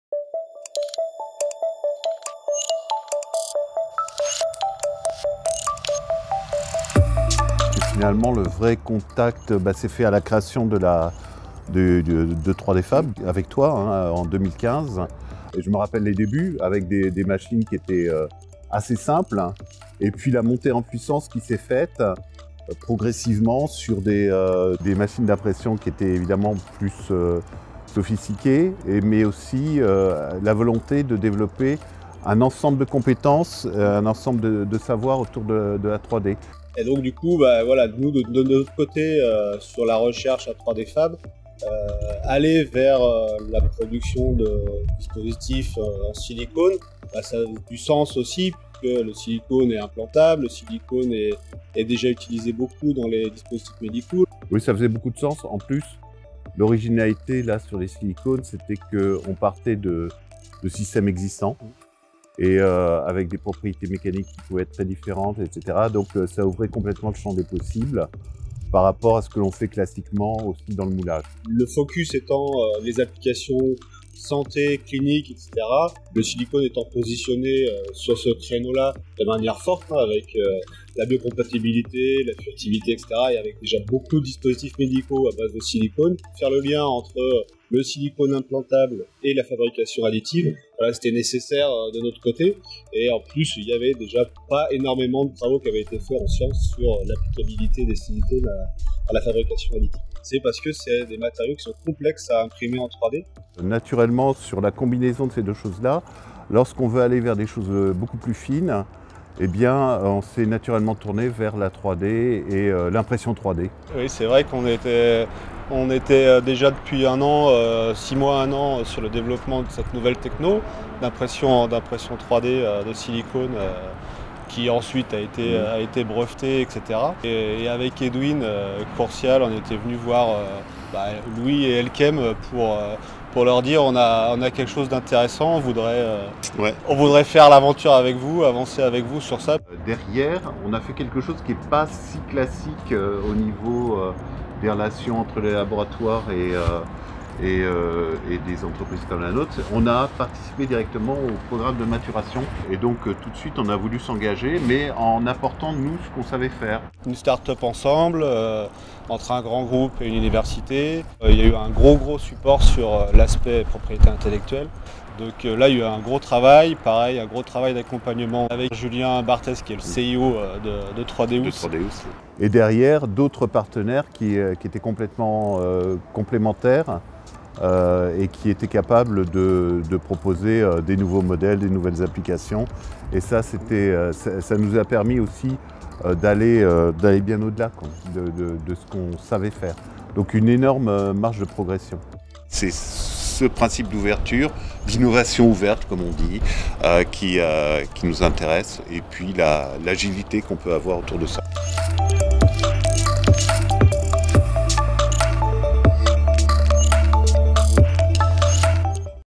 A partnership with 3Deus Dynamics
Discover the genesis of one of our initiatives with 3d.FAB platform, the only French "Innovative Technological Platform" based at Lyon Claude Bernard University. For years, Elkem Silicones has maintained strong collaborations in Open Innovation with a diversity of partners such as academic laboratories, universities, industrial clusters, ...

Learn more about the example with 3Deus Dynamics.